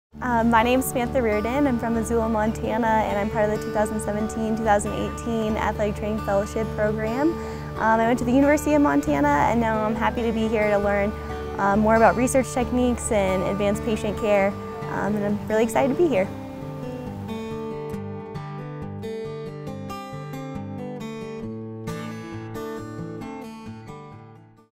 Athletic Training Resident Sam Riordan
The Steadman Clinic welcomes Sam Riordan, one of our Athletic Training Residents for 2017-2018!